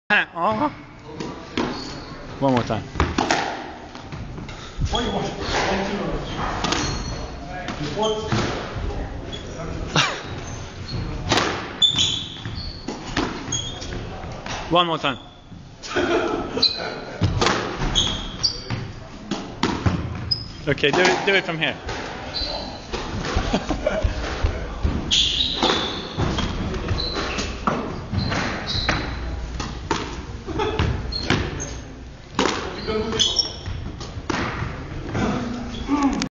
The frog
C Labite visar sina tricks på banan.
Släpper bollen förbi sig på backhand och slår sedan bollen med forehand till främre backhandhörnet